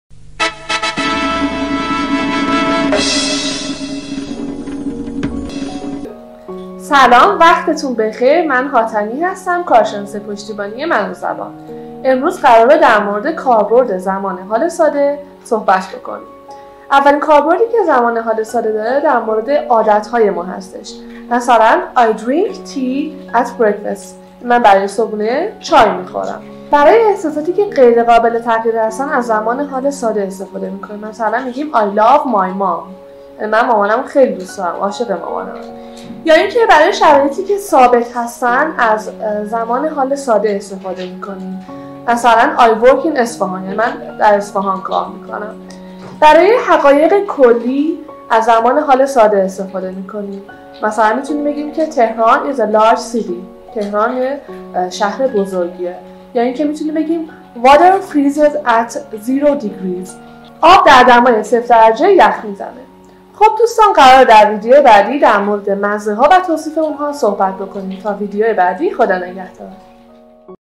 کاربردهای حال ساده در زبان انگلیسی
اولین زمانی که در انگلیسی یاد می گیرید، زمان حال ساده است. از این زمان می توان در موقعیت های زیر استفاده کرد:
 عادت ها: I drink tea at breakfast (صبح ها چای میخورم)
احساسات غیر قابل تغییر: I love my mom (مامانم رو خیلی دوست دارم)
شرایط ثابت: I work in Isfahan (من در اصفهان کار می کنم)
حقایق کلی: Tehran is a large city (تهران شهر بزرگی است)
آموزش زبان انگلیسی | manozaban.ir